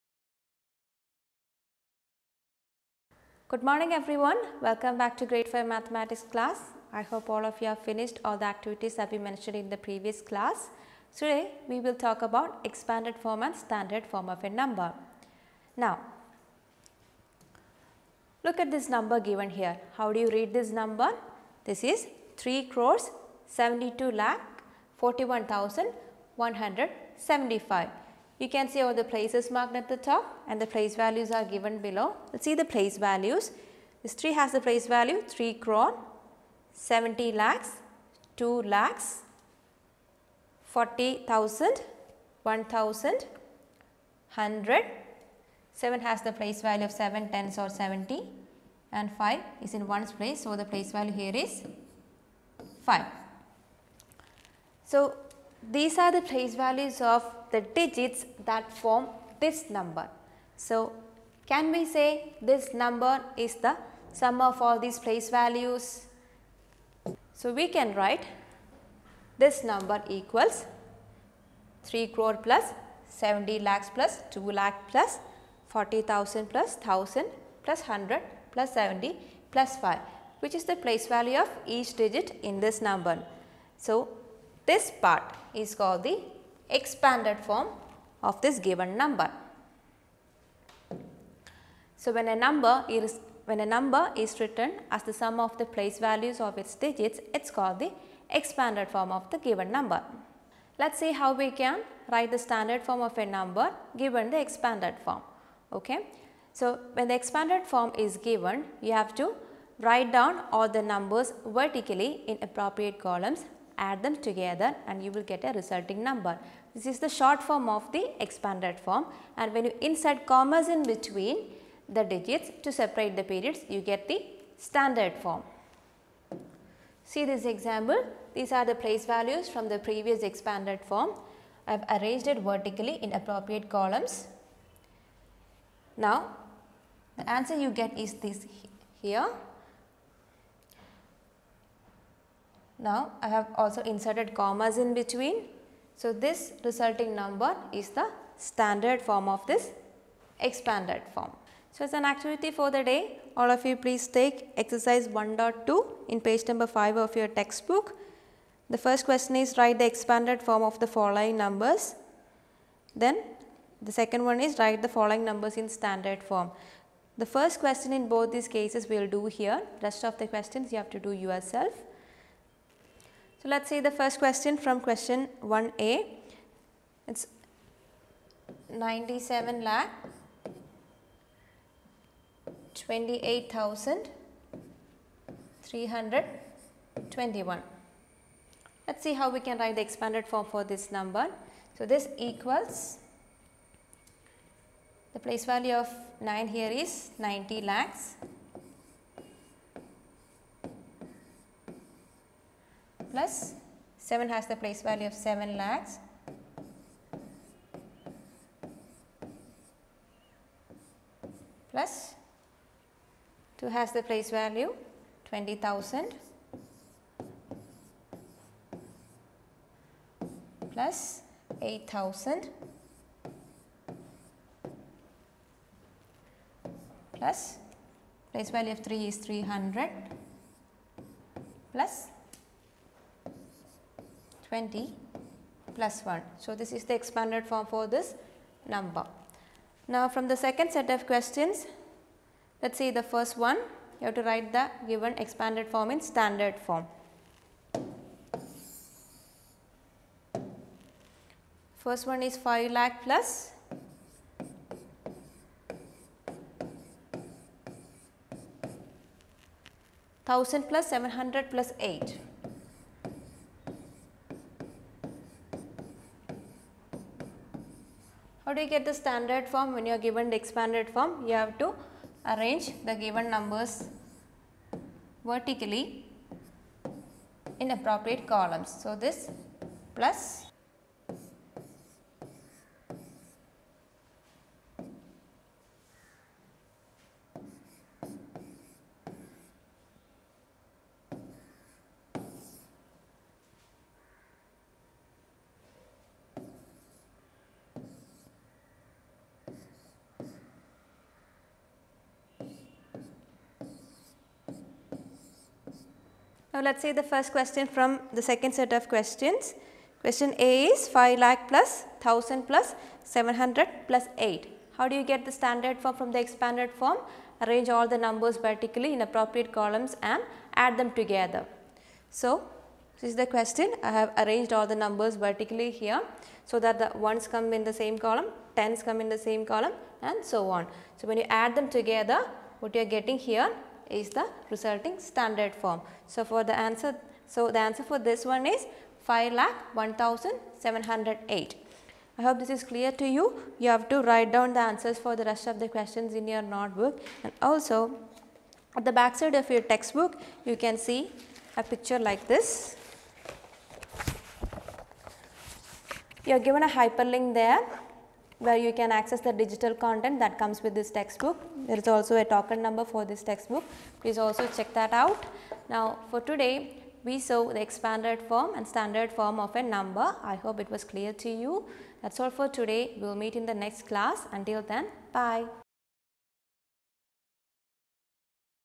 Grade 5 Mathematics Expanded and standard form
Online classrooms for Grade 5 students of The Garden School. Subject: Mathematics.